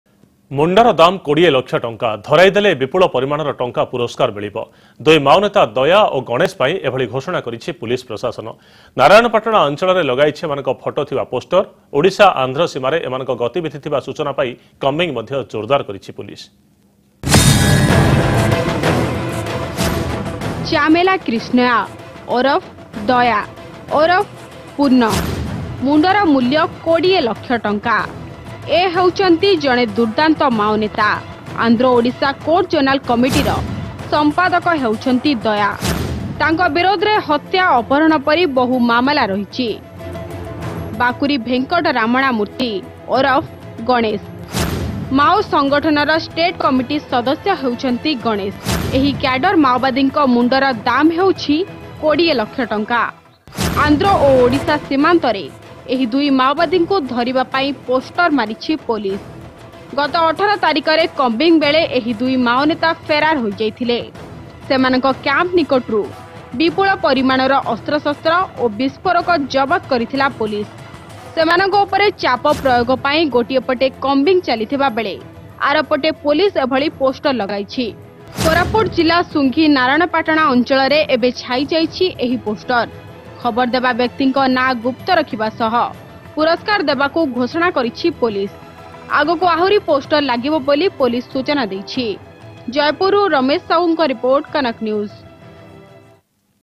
Rs 20 Lakhs Reward On Maoists
Police have kept Rs 20 lakhs cash rewards on two Maoists leader Chamella Kirstnaiah aka Daya and Bakuri Venkata Ramana Murty